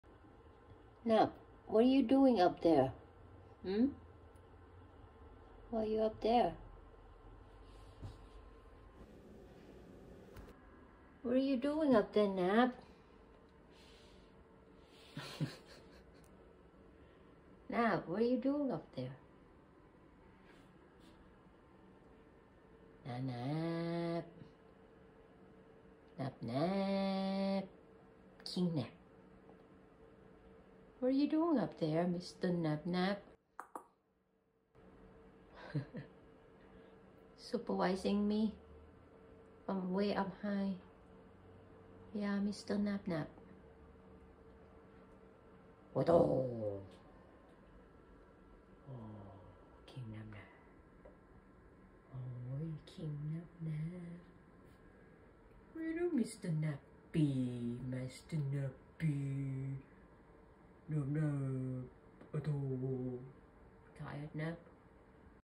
I’m supervising my human doing dishes from up here😸🤭
Napnap used to be up on the top of cabinets a lot when he is younger. 😻